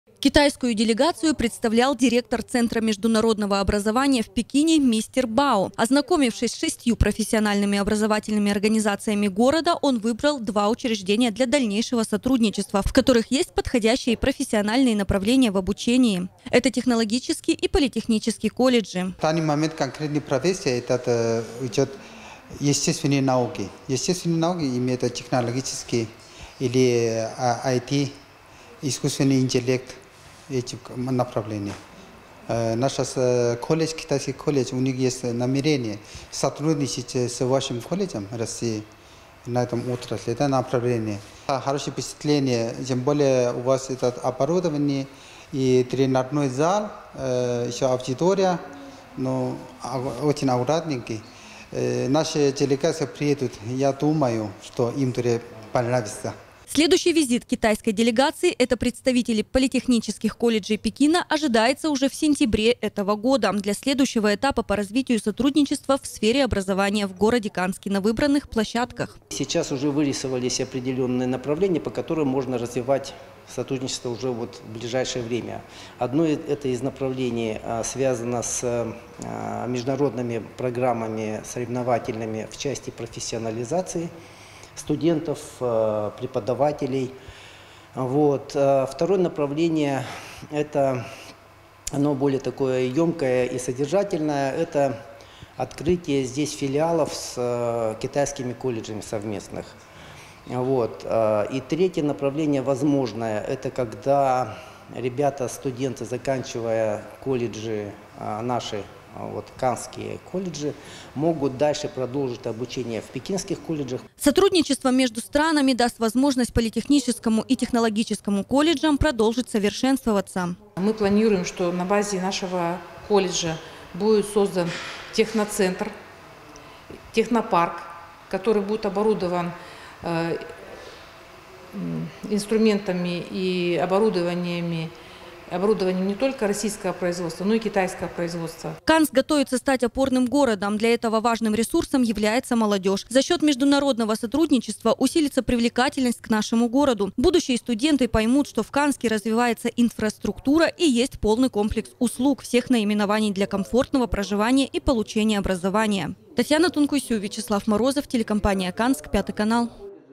КИТАЙСКАЯ ДЕЛЕГАЦИЯ
Накануне делегация из Пекина посетила Канск и побывала на экскурсиях в профессиональных образовательных учреждениях нашего города.